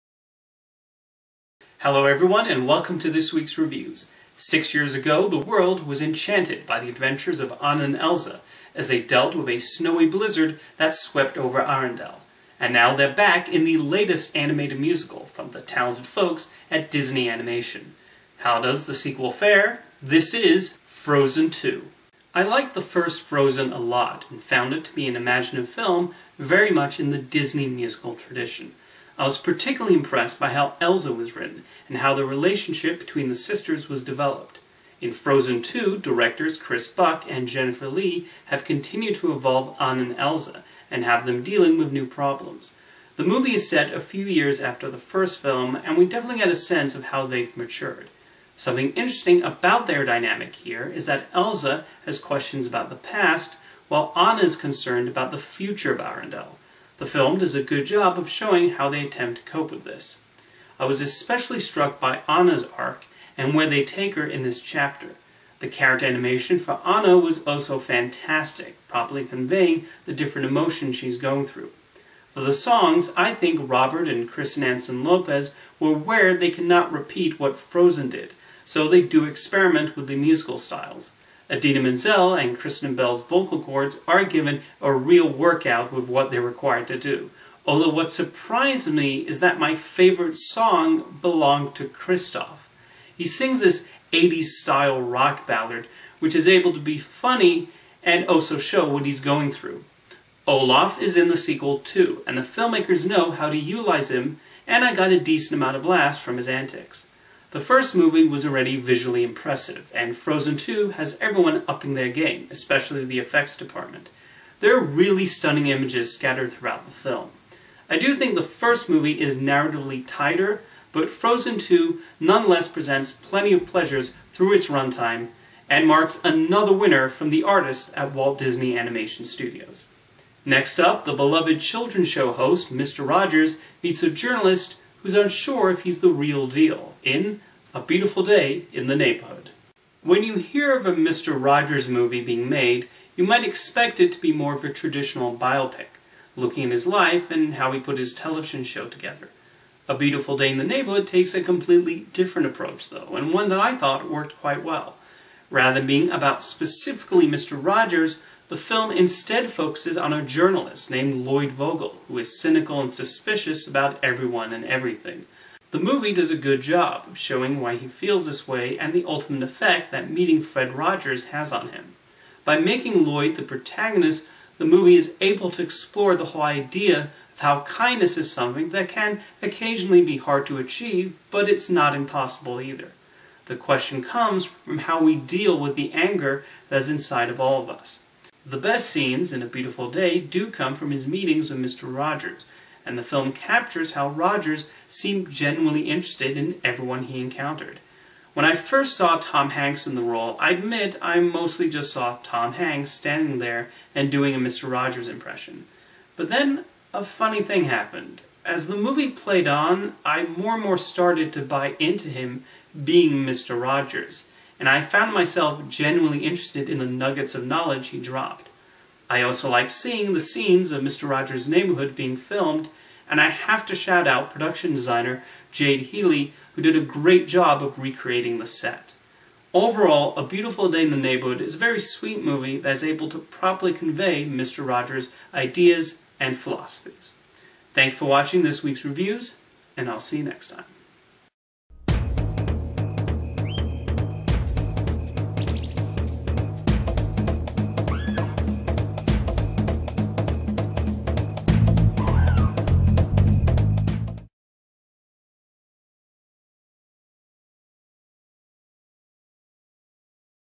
Mr. Coat's Movie Reviews: "Frozen II" and "A Beautiful Day in the Neighborhood"
This week, I review Disney Animation's musical sequel to their blockbuster hit "Frozen II" and the Mr. Rogers-inspired movie "A Beautiful Day in the Neighborhood."